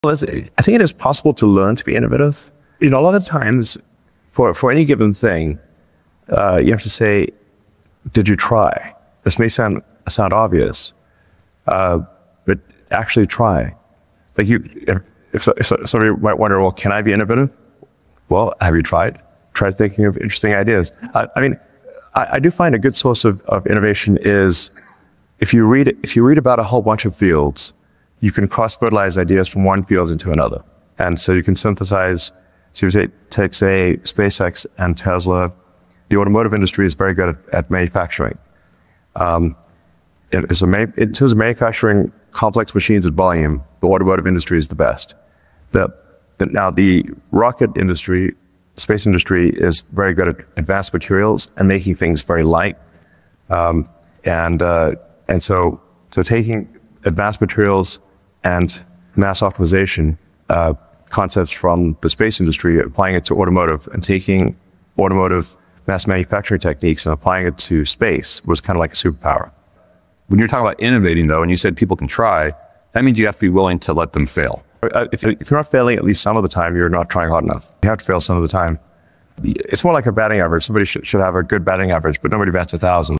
Unlock Your Inner Innovator: Lessons from Elon Musk!
Innovation isn't just for the selected few; it's a skill that can be nurtured and grown. Discover how embracing failure can pave the way to groundbreaking ideas. Dive into the insights that blend automotive excellence with aerospace advancements, showing how cross-disciplinary thinking fuels creativity. This engaging discussion reveals the importance of trying, failing, and evolving in the pursuit of innovation. Let the experiences shared from the Tesla journey inspire you to think outside the box and transform your ideas into reality.